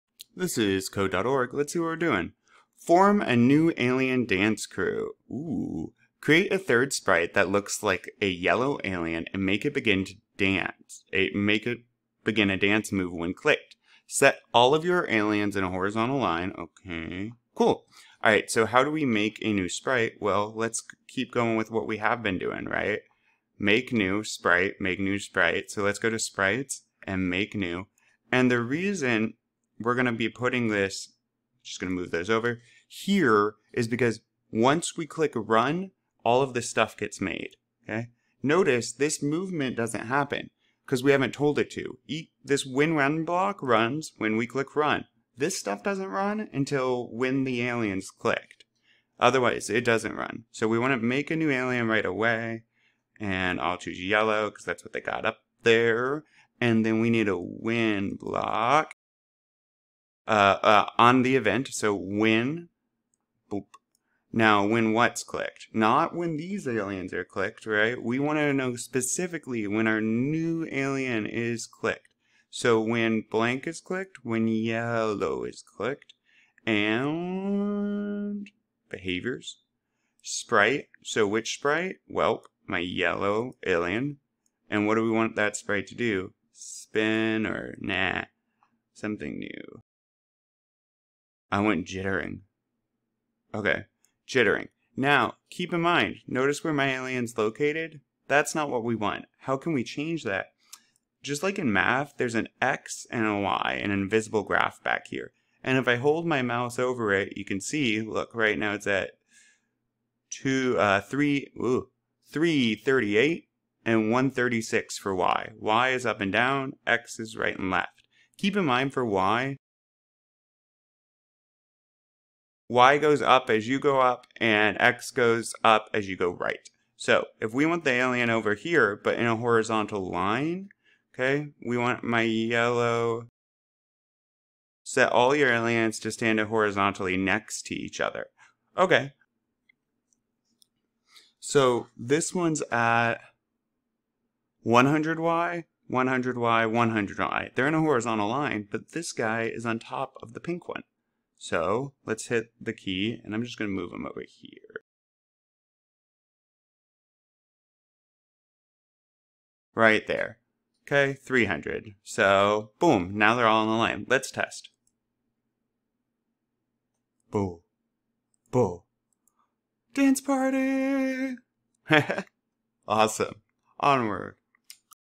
Code.org Alien Dance Party with Sprite Lab Part 5 Answers | Course E & F Lesson 7 & 3 Express 25
Alien Dance Party is lesson 25 of the Express Course, lesson 7 of Course E, and lesson 3 of Course F from Code.org's C.S. Fundamentals curriculum. This course is an excellent way to learn to code. Each part challenges students to accomplish a new task while learning computer science concepts. This particular course,  C.S. Fundamentals is often used in elementary. In this video, I walk through step-by-step how to get the right answer for this puzzle. Tutorials for more lessons are linked below.